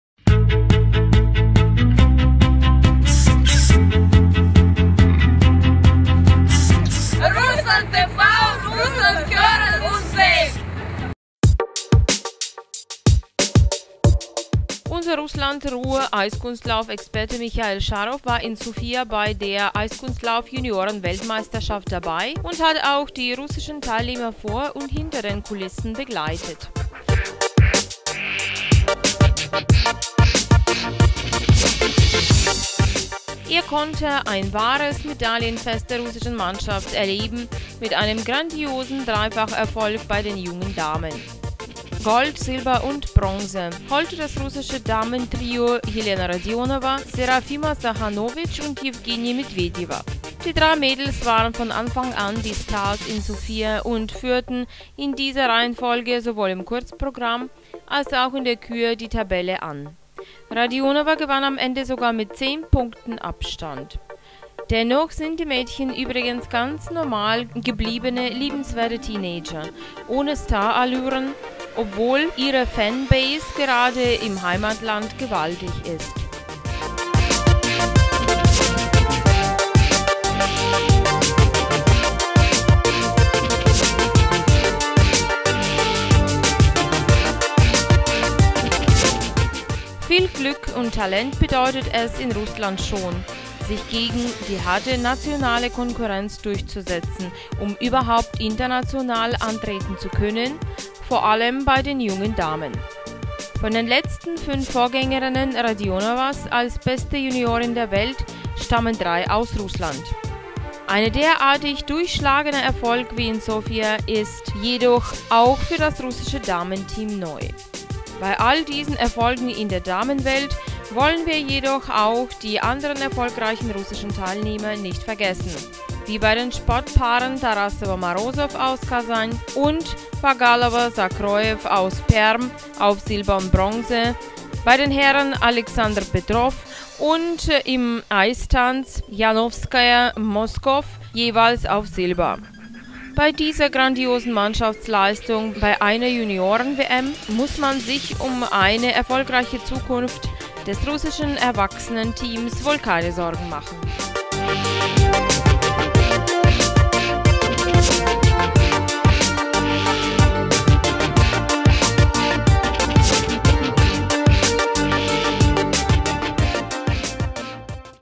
Eiskunstlauf Junioren-WM
Da unser Eiskunstlauf-Experte Michail Scharow nicht nur mit Stift und Fotokamera, sondern auch mit einer Filmkamera in Sofia bei der Junioren-WM in Sofia war, hier nun auch unser Video über diese Meisterschaft, die Russland eine regelrechte Medaillenflut bescherte. Einmal Gold, dreimal Silber und zweimal Bronze gab es für die russische Mannschaft, darunter einen Dreifach-Erfolg bei den Damen durch die Titelverteidigerin Elena Radionowa (Gold), die russische Juniorenmeisterin Serafima Sachanowitsch (Silber) und Jewgenija Medwedjewa (Bronze). Aber auch all die anderen Medaillengewinner sind bei unserem Blick hinter und vor die Kulissen der Meisterschaft in Sofia zu sehen. Denn Edelmetall gab es in jeder Disziplin.